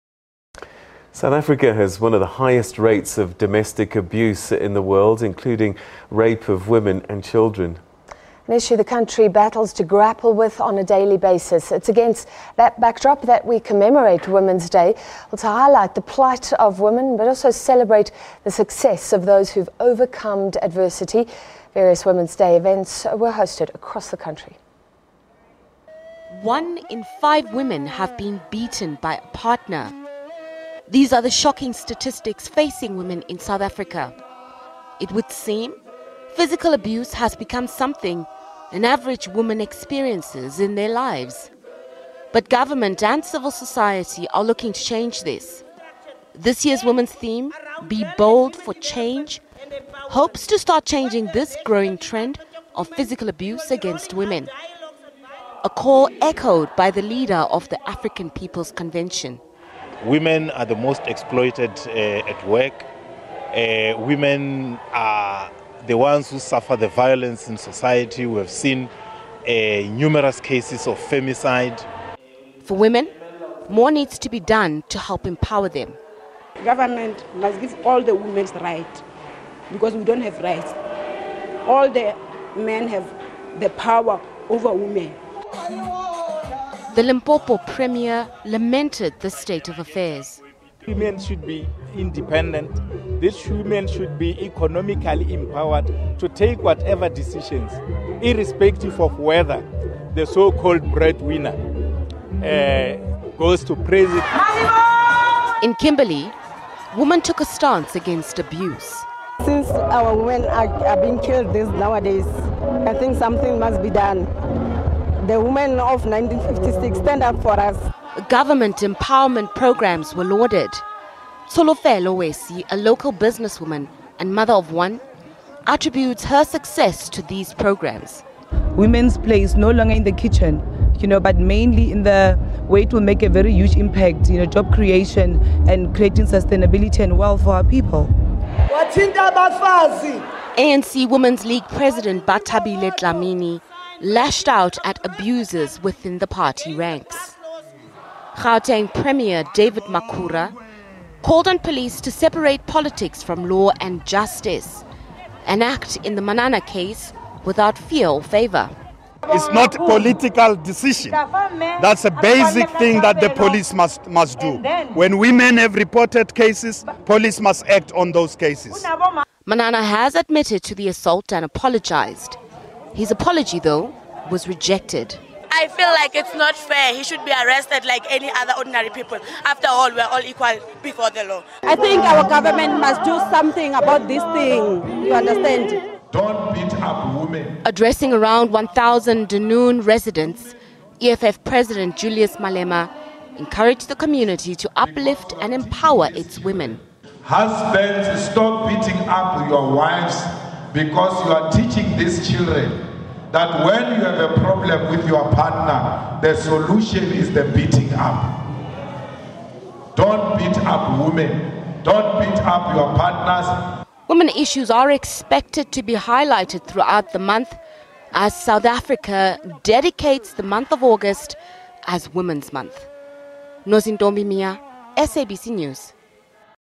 WRAP: Women's Day 2017 commemorated across the country
South Africa has one of the highest rates of domestic abuse in the world including rape of women and children. Issue the country battles to grapple with on daily basis. It's against this backdrop that we commemorate women's day. To highlight the plight of women but also celebrate the successes of those who've overcome adversity. Various Women's day events were hosted across the country.